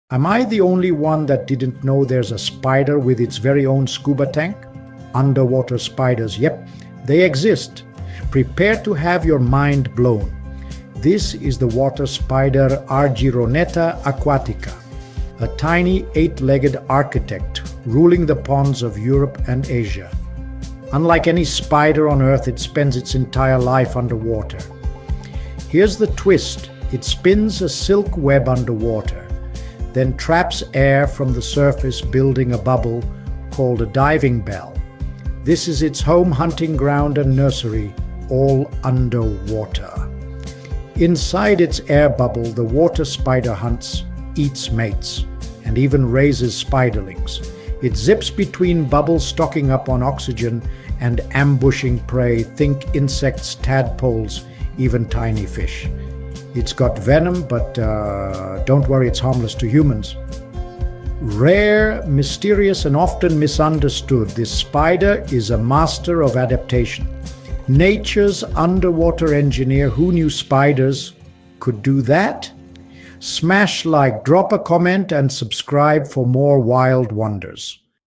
🕷️ Water Spiders Masters of the Underwater Air Bubble 🌊💨
Meet the Water Spider (Argyroneta aquatica), nature’s aquatic architect! Unlike any other spider on Earth, this fascinating creature lives entirely underwater by creating its own air-filled bubble “diving bell.” Found in freshwater ponds and lakes across Europe and Asia, the water spider spins silk webs underwater, trapping air from the surface to build its unique, submerged home. 🕸️🌬️

🧠 Mind-Blowing Water Spider Facts:
✅ Underwater Lifestyle – The only spider that lives and hunts underwater full-time! 🌊🕷️
✅ Air Bubble Home – Builds a diving bell made of silk to trap and store breathable air. 🫧🏠
✅ Aquatic Predator – Feeds on insects, tadpoles, and small fish while remaining submerged. 🐟🪰
✅ Venomous Bite – Uses venom to paralyze prey—but it's harmless to humans. ⚠️
✅ Rare & Mysterious – A unique species that’s rarely seen and often misunderstood. 🔍

📍 Where to Find Them?
Mostly found in clean, still freshwater across Europe and northern Asia, often hiding among aquatic plants. 🌾💦

🎥 Dive into the world of the Water Spider! Learn how this tiny arachnid became an underwater engineer. 🌍🔔